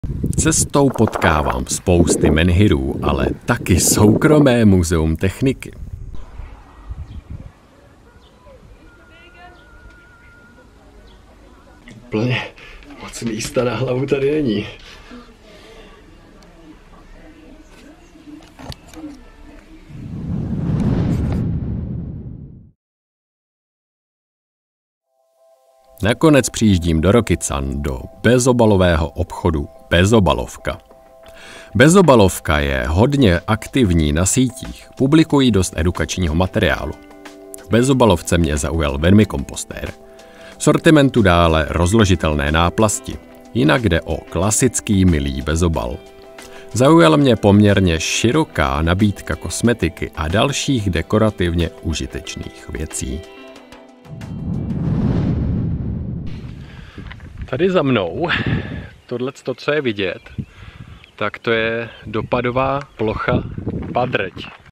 Rokycany - bezobalový obchod Bezobalofka
Výprava Aš na Kremenec. To je cesta na koloběžce Mibo Split po 17 bezobalových obchodech. Bezobalofka (Rokycany), je dalším zastavením na cestě.  🌱

👉 Přihlas se k obsahovému eko zpravodaji. První neděli v měsíci si smlsneš na zajímavostech, které mě cestou k nule (odpadu) cvrnkly do nosu

 kdy jsem se vydal na cesty na koloběžce 🛴 Mibo Mastr,  na 800 km dlouhou cestu po bezobalových obchodech po Česku.